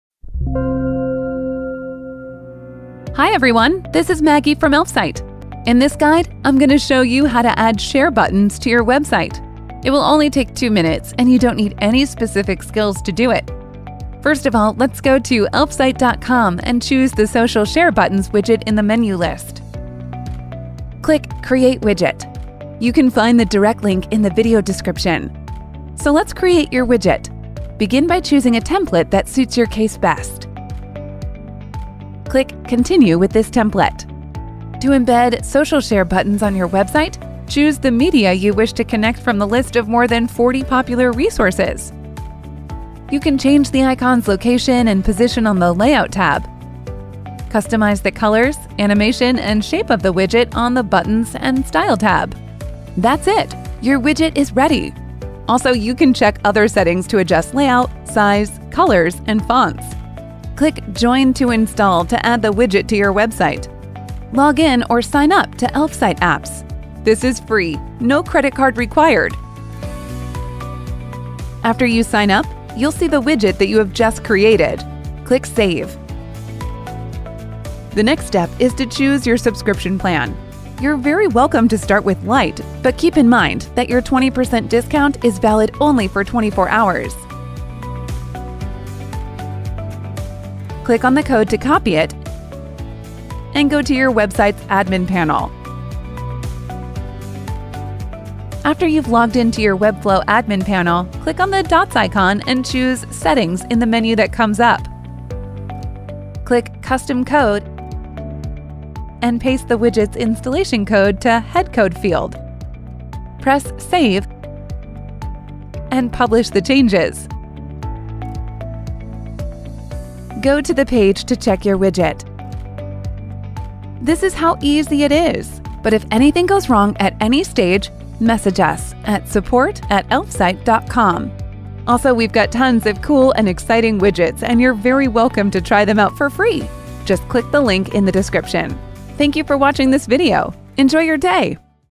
How to Add Social Share Buttons Widget to Webflow
Add Webflow Share Buttons widget to website in just 2 minutes -

Social Share Buttons widget is the best free tool to choose unlimited number of share buttons and spread your content on all the most popular platforms. Reach more of your target audience and enhance your brand in social networks.

Features:

• Customizable style of the media icons.
• Opportunity to select one of the 8 layouts.
• List of forty predefined social buttons.
• Any type of predefined button shapes: Rounded, Circle, or Rectangle.

How to embed Social Share Buttons on a Webflow website?

In order to add our plugin, view the steps from the brief guide below:

1. Create and customize the widget;
2. Copy your personal code which you will see in the popup;
3. Embed the received code on the website.

Follow the link below in order to find out how to add Webflow Social Buttons plugin -

Best Share Buttons widget examples:

Example 1: Inline buttons - the most common variant of the plugin where buttons are arranged horizontally.
Example 2: Floating Icons - this layout can be positioned by any edge of the screen and will chase the scroll all the time
Example 3: Black Icons - black and white colour palette makes this template truly special.

Here’s how comfy it is to add Social Buttons plugin to Webflow! If you wish to learn more about features, widget templates, and configs, you are free to write to our competent Support specialists. We’ll be glad to answer all your questions!